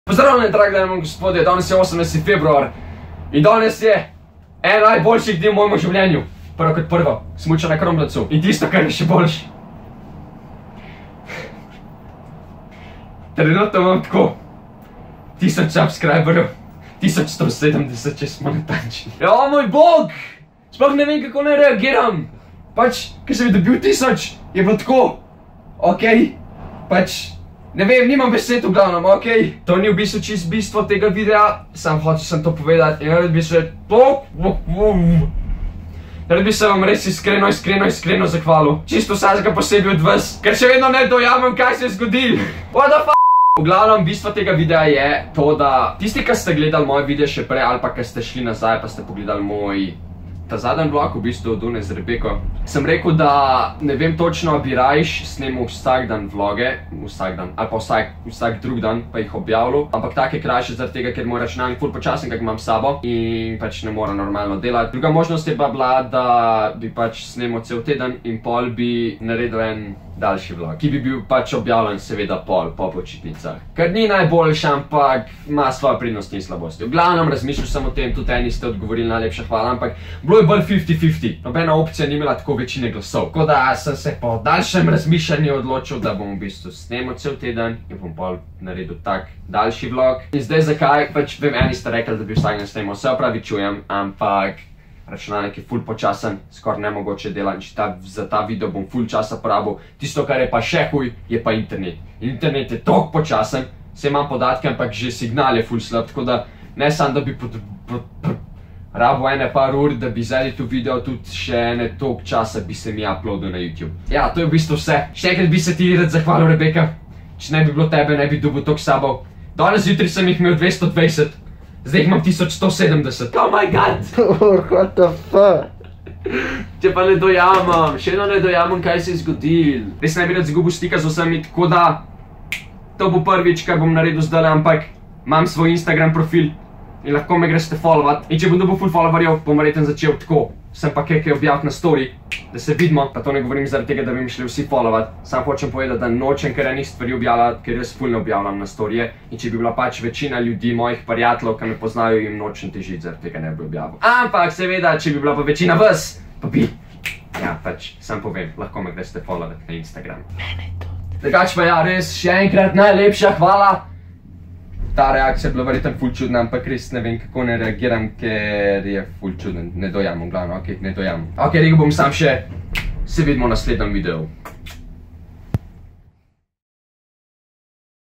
Update video - Ni vlog
-ta video bo zbrisan takoj k bo naslenji vlog objavln-

Razn tega, da je biu dons najboljši dan v mojem življenju, je biu tut najbl zmeden in še vedno sem tut sam zmeden ker se je tok enih stvari zgodil v zlo mejhnm času. In spet kr neki nakladam (točno o tem govorim). To sm reku že 100x ampak niti 100x ni dost, ker vs je zdej več k 1000, velik več k tisoč in tolkrat hvala ne bom mogu nikol izgovort. Ampak vseen. NAJLEPŠA HVALA!!♥ Res ste nrdil moje življenje lepše in nikol ne bom pozabu na to. Seveda pa ne morm spet ne oment tebe, Rebeka... Nimam besed, prsežm da ne.

V glavnem upam da ste razumel kaj sm hotu povedat, pa tut tok da veste... Vlog ne bo čist takoj po počitnicah objavln ker mam tist tedn 2 testa, se bom pa potrudu da bo čim prej.

HVALA še zadnič (dons) in pa, kot vedno... Se vidmo v naslednjem videu;D

Peace.

P. S.
Za editanje tega videa sm porabu 3 ure. In kljub temu je zanč. xD